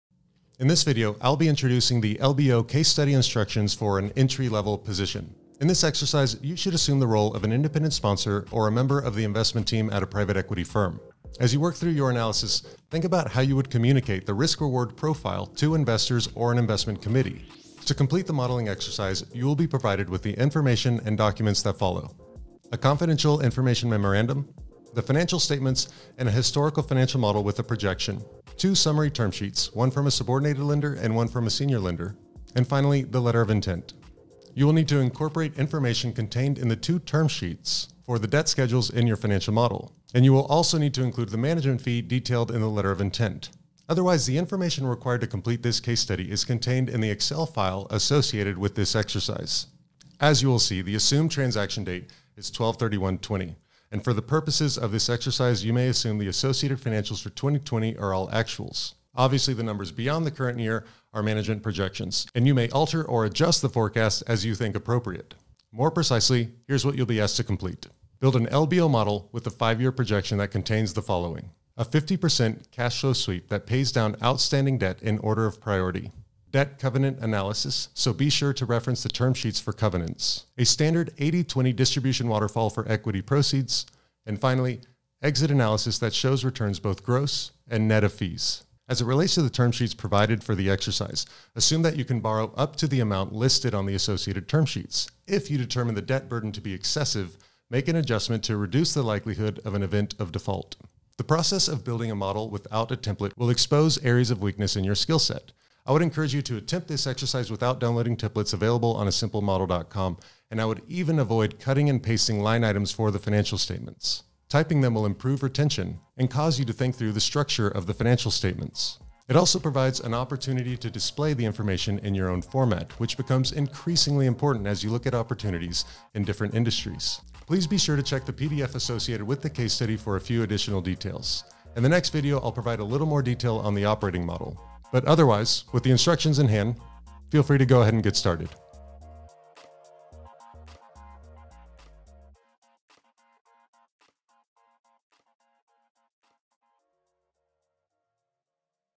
LBO Case Study Instructions | Entry Level Position in Private Equity
Test your LBO financial modeling skill set for an entry level position at a private equity firm.

Developed by Industry Professionals:
This case study was developed as part of a content creation initiative with Katten, a law firm with 600+ lawyers. I have worked on private equity transactions with partners that are now at Katten for most of my professional career.